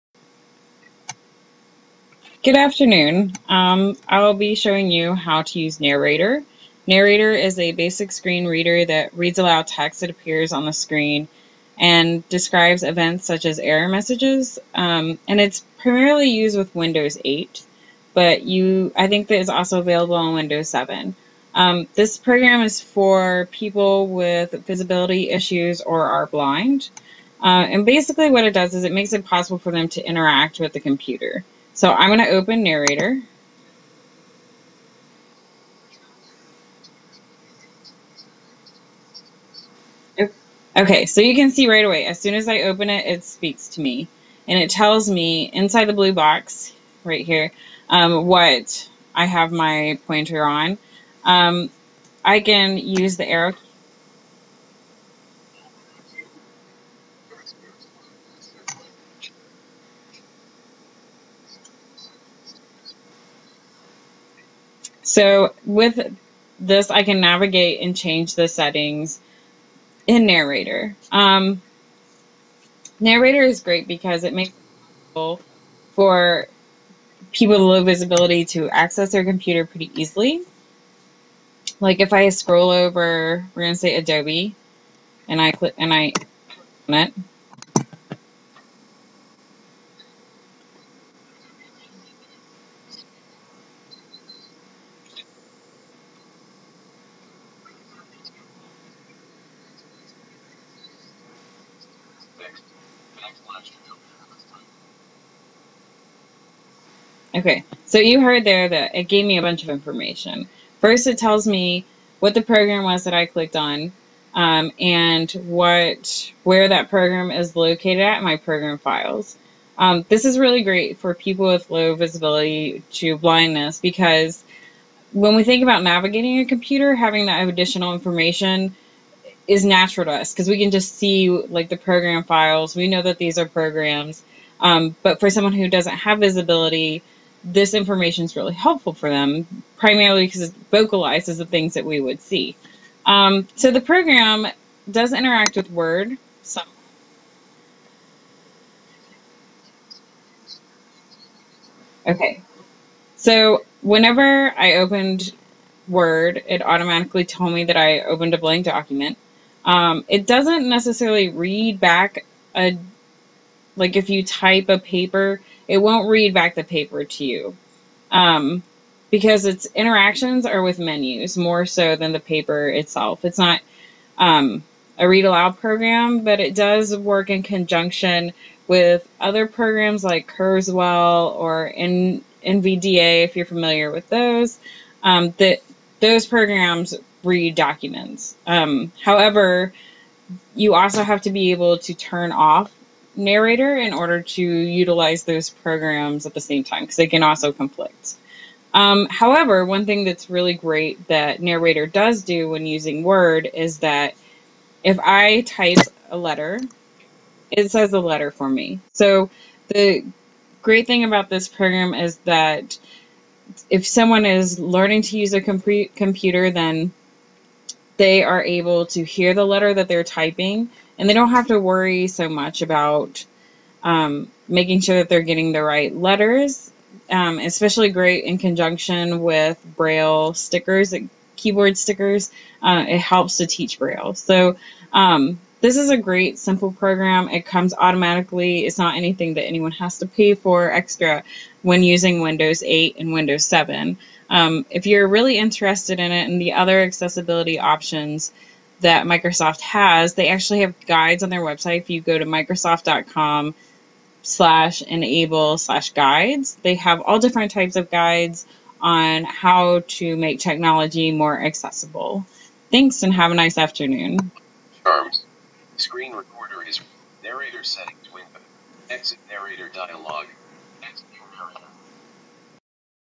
Using Narrator in Windows 8
This video describes the basic use of narrator in Windows 8. It is difficult to hear the screen reader in the video because of the recording